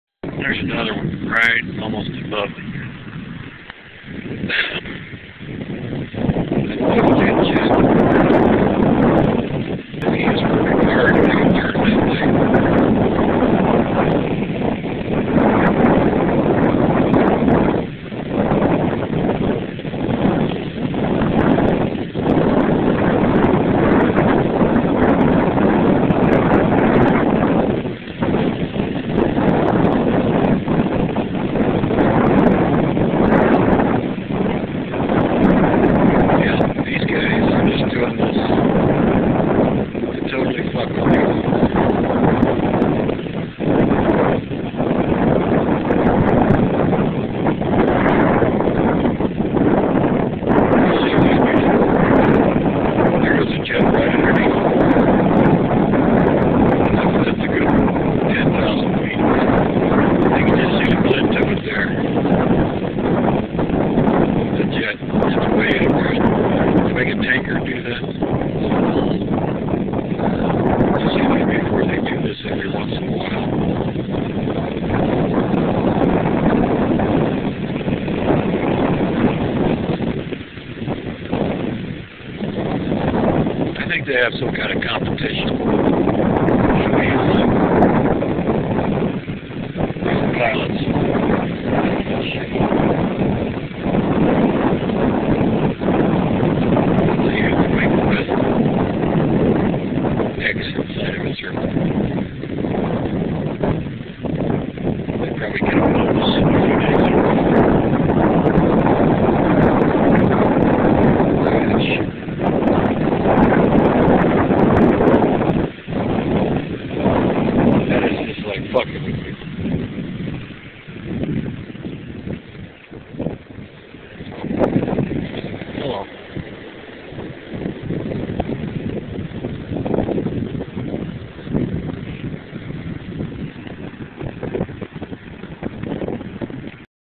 X Men advertising convenient cover for more chemical poisoning
Wow, cool huh... jet tankers at 20,000 to 30,000 feet making big billboards in the sky! Interesting these things are at the same altitude and the same physical makeup as all those chemtrails you deny seeing or believing. If you're a dumb kid or a braindead minion over the age of 30 you don't remember a clear, deep blue sky without all these odd cloud trails behind all these nondescript tanker jets. So go see the next simpleton movie and be sure to eat the gmo popcorn with lots of corn syrup and chemicals like Splenda (sucralose), Sweet'N Low (saccharin), Equal and NutraSweet (aspartame), acesulfame K and neotame "butter" on it.